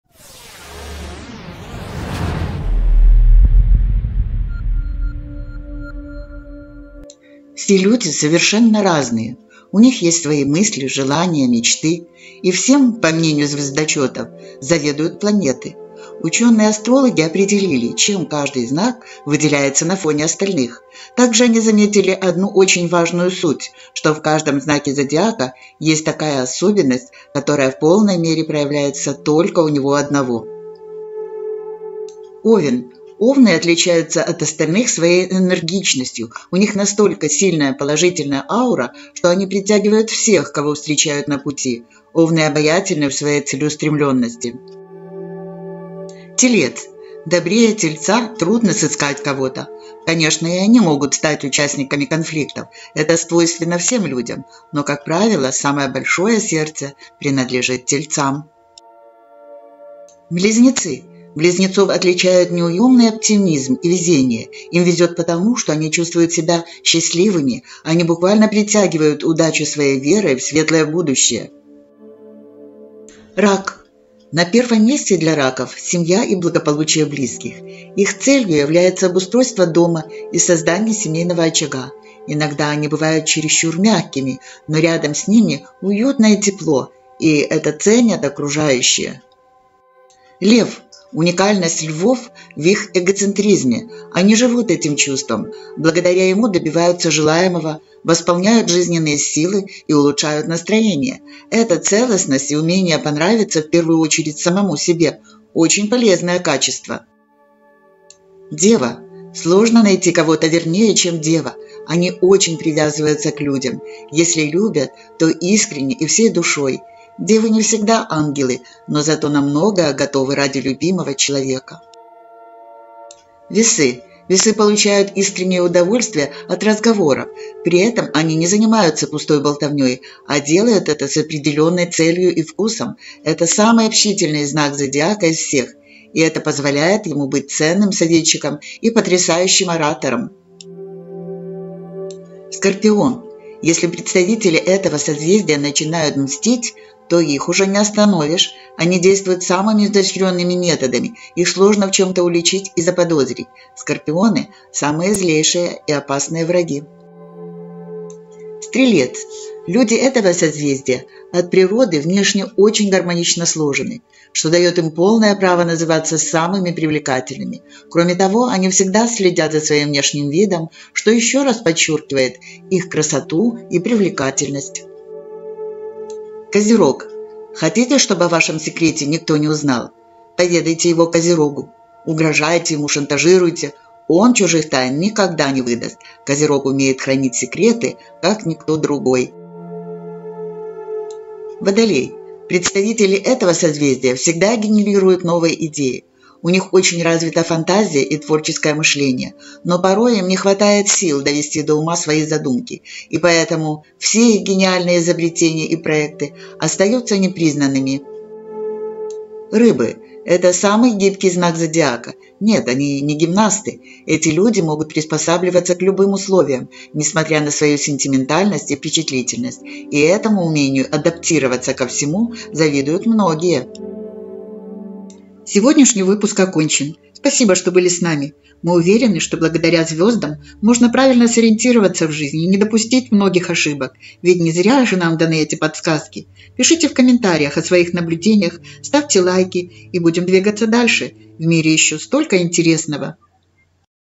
ЧЕМ УНИКАЛЕН ВАШ ЗНАК ЗОДИАКА
Все люди совершенно разные. У них есть свои мысли, желания, мечты, пишет Marketium. И всем этим, по мнению «звездочетов» заведуют планеты. Ученые-астрологи определили, чем каждый знак выделяется на фоне остальных.

Также они заметили одну очень важную суть, что в каждом Знаке Зодиака есть такая особенность, которая в полной мере проявляется только у него одного.

Овен (0:31)
Телец (0:46)
Близнецы (0:59)
Рак (1:13)
Лев (1:30)
Дева (1:48)
Весы (2:03)
Скорпион (2:22)
Стрелец (2:38)
Козерог (2:56)
Водолей (3:11)
Рыбы (3:29)